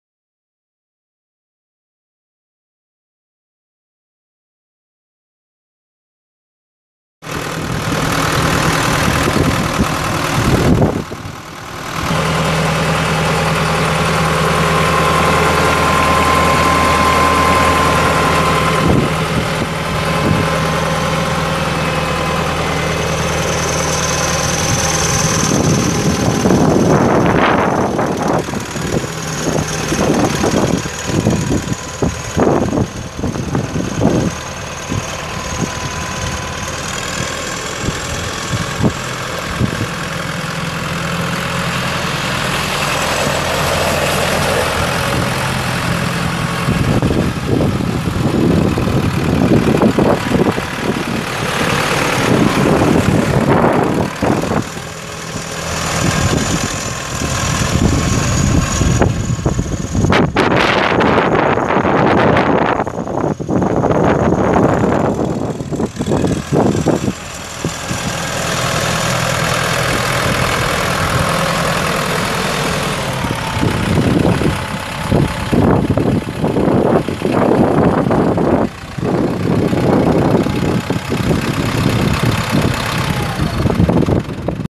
1999 New Holland TS100 tractor for sale | no-reserve Internet auction March 21, 2017
1999 New Holland TS100 tractor
4,821 hours on meter
Ford four cylinder diesel engine
72 HP
Synchro transmission
12F-12R gears
AC and heat
AM/FM
Shades
Three point
Unattached but included
540 PTO
Seven pin outlet
ISO electrical outlet
(6) rear hydraulic remote sets
(2) capped
Rear window guard
93" wheelbase
Front axle 93"W
Rear axle 100"W
11L-16SL front tires
18.4-30 rear tires
Battery dead
Paint faded and scratched
Serial 135608B